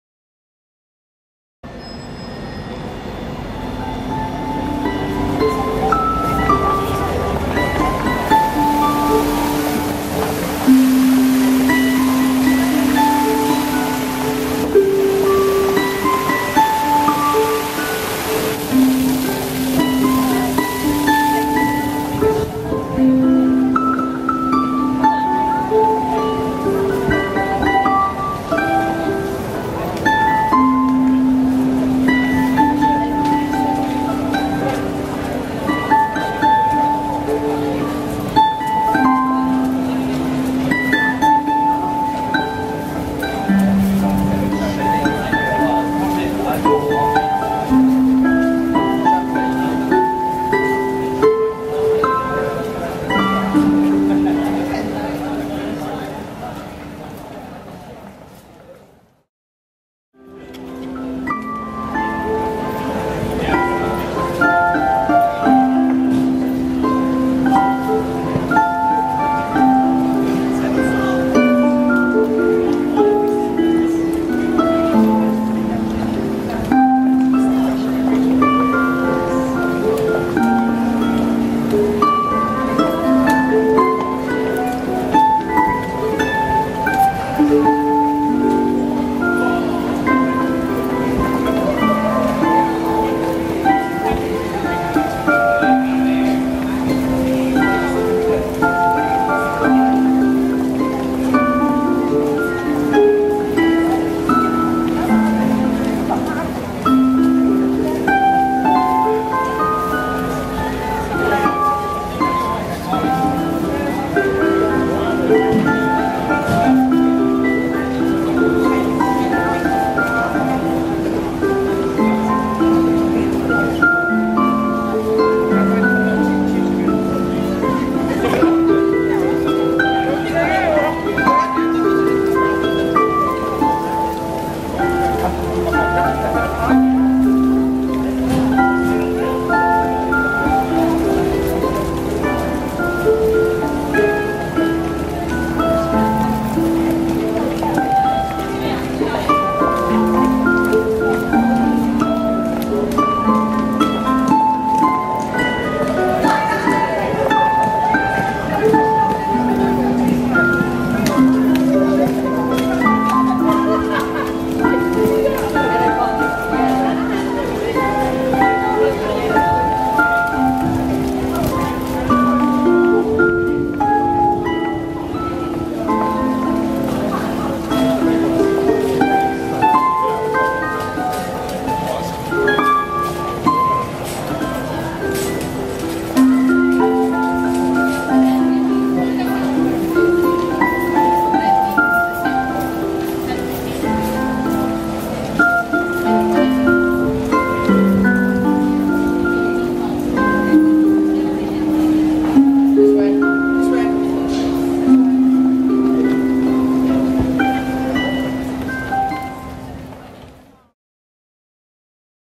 Street harpist Christine in Nottingham. 1080HD.
Harpist Christine Palethorpe brings tranquility and delight to shoppers on Clumber Street, Nottingham, this afternoon as she played on her Lap Harp.